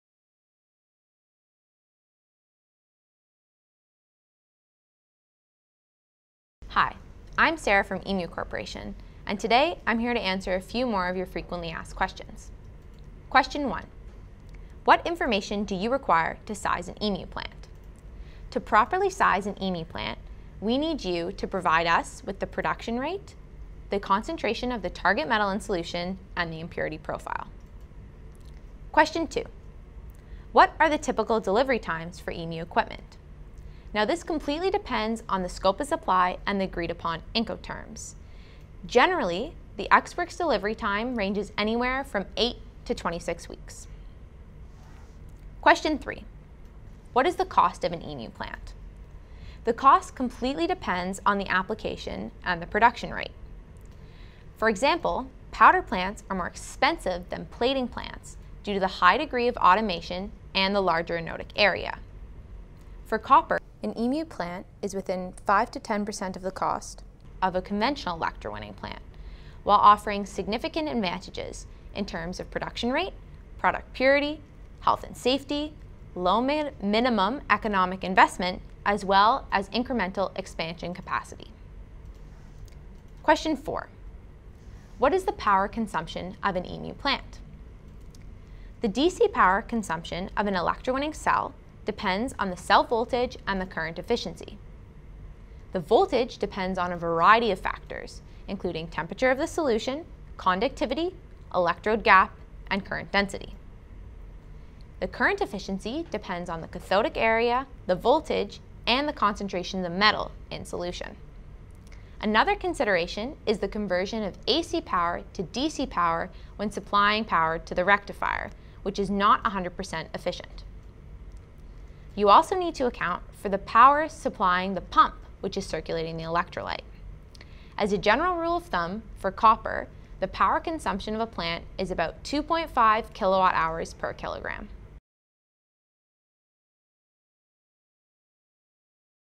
FAQ: Operating Cost of emew Electrowinning Plant
In this video we answer questions about how to size your emew plant, what are typical delivery times for emew equipment, and what is the cost of emew plant.